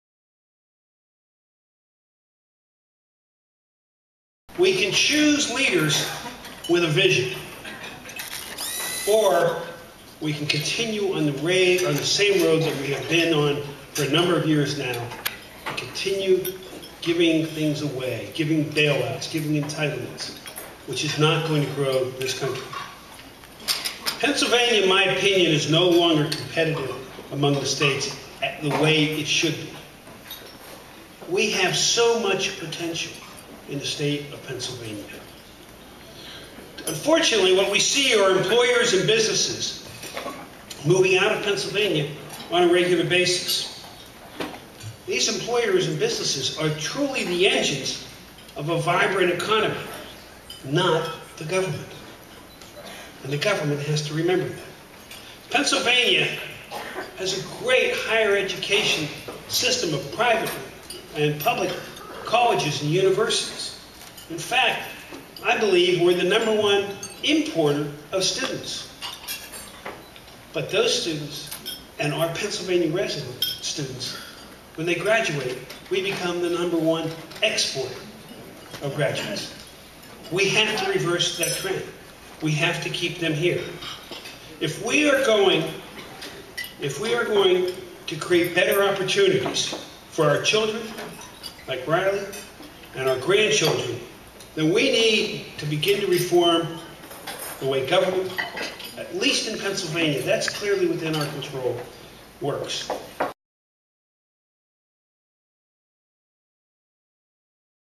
Tom Corbett at the Pennsylvania Leadership Conference
Pennsylvania Attorney General Tom Corbett, a likely GOP candidate for governor, addresses conservatives at the Pennsylvania Leadership Conference in March. Visit pa2010.com for all the latest news, analysis and commentary on Pennsylvania politics.